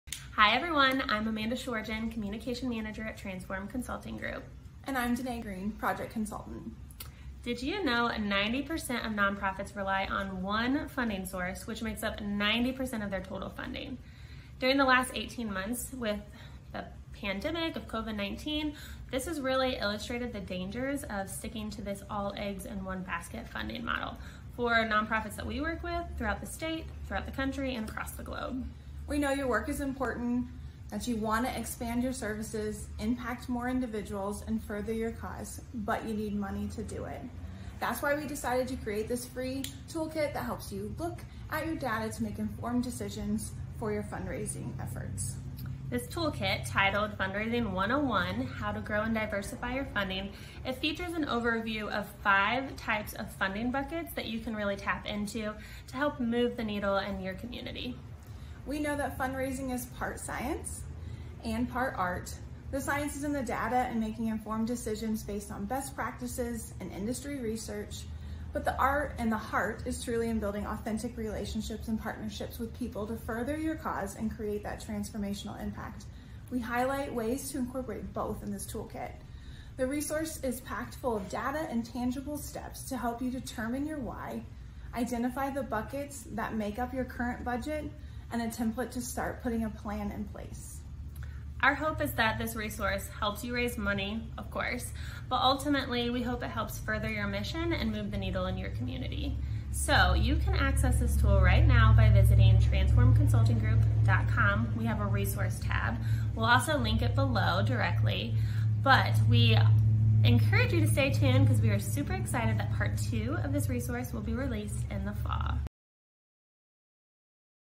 Fundraising Toolkit 101 Overview
Did you know 90% of nonprofits rely on one funding source which makes up 90% of their total funding?

COVID-19 illustrated the dangers of an “eggs-in-one-basket” funding model for nonprofits across the globe.

We’re passionate about helping you accelerate your impact, and we understand that building your capacity usually comes back to what funding is available. We created a toolkit that highlights 5 funding streams to consider and tangible tips for tapping into new, diverse funding options to reach your fundraising goal!